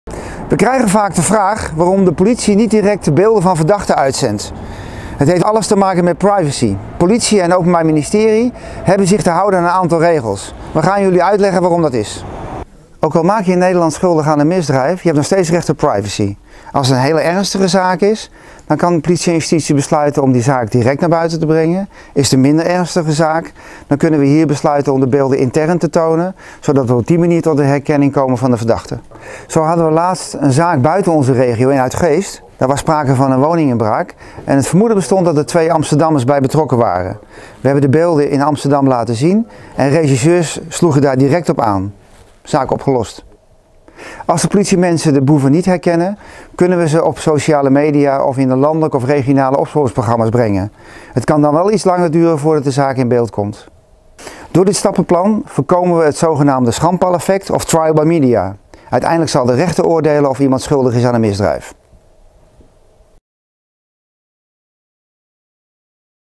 Videobeelden in opsporingsonderzoek delen met media
In deze video leggen we je uit wanneer we videobeelden delen met (social) media om te achterhalen wie verantwoordelijk is voor bepaalde misdrijven. De privacy wordt daarbij door regelgeving zo veel mogelijk gewaarborgd.